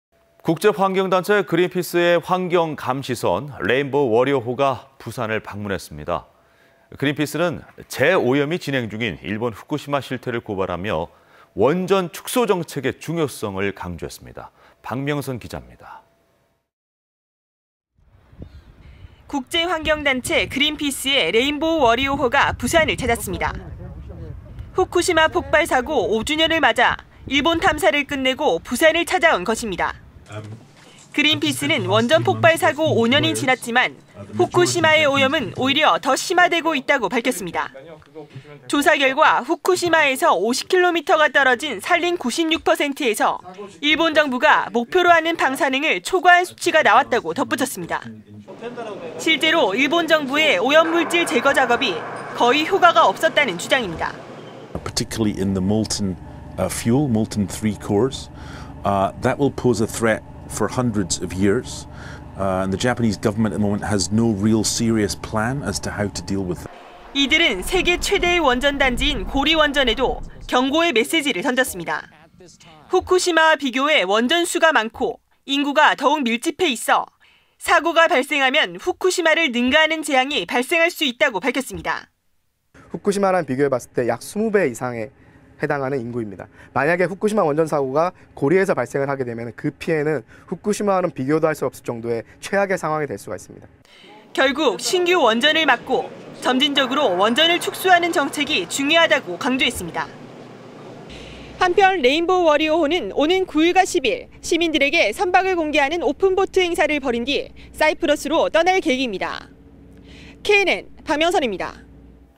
[KNN 뉴스] 그린피스 후쿠시마 일부정부 설명보다 오염 심각
{앵커:
국제환경단체 그린피스의
환경감시선 레인보우 워리어호가
부산을 방문했습니다.

그린피스는 재 오염이 진행중인 일본
후쿠시마의 실태를 고발하며
원전 축소 정책의
중요성을 강조했습니다.

박명선 기자가 보도합니다.}

{리포트}

{수퍼:그린피스 레인보우 워리어호/오늘(7), 부산항}
국제 환경단체 그린피스의 레인보우 워리어호가 부산을 찾았습니다.

후쿠시마 폭발사고 5주년을 맞아
일본 탐사를 끝내고 부산을 찾은
것입니다.

그린피스는 원전 폭발사고 5년이
지났지만 후쿠시마의 오염은 오히려 더 심화되고 있다고 밝혔습니다.

{수퍼:'후쿠시마 오염은 더 심화'}
후쿠시마에서 50km 떨어진 산림 96%에서 일본 정부가 목표로 하는 방사능을 초과한 수치가 나왔다고 덧붙였습니다

일본 정부의 오염물질 제거 작업이
거의 효과가 없었다는 주장입니다.

{인터뷰:}
{수퍼:숀 버니/그린피스 독일 사무소 원전 전문가/"앞으로 수백년동안 더욱 많은 방사능 오염 물질이 지속적으로 나올 것입니다. 하지만 일본 정부는 이 상황을 해결할 수 있는 어떤 대책도 없습니다."}

이들은 세계 최대의 원전단지인
고리원전에도 경고의 메세지를
던졌습니다.

{CG:}
후쿠시마와 비교해 원전 수가 많고
인구가 더 밀집해있어  사고가 나면 후쿠시마를 능가하는 재앙이
발생할 수 있다고 밝혔습니다.

{인터뷰:}
{수퍼:장다울/그린피스 동아시아 서울사무소 캠페이너/"(고리원전의 경우) 후쿠시마와 비교했을때 약 20배 이상 해당하는 인구가 있습니다. 만약 원전사고가 고리에서 발생한다면 그 피해는 후쿠시마와 비교도 할 수 없는 최악의 상황이 될 수 있습니다."}

{수퍼:영상취재 국주호}
결국  신규 원전을 막고 점진적으로 원전을 축소하는 정책이 중요하다는
점을 강조했습니다.

{StandUp}
{수퍼:박명선}
"한편 레인보우 워리어호는
오는 9일과 10일 시민들에게
선박 내부를 공개하는 오픈보트 행사를 벌인뒤 사이프러스로 떠납니다.

KNN 박명선입니다."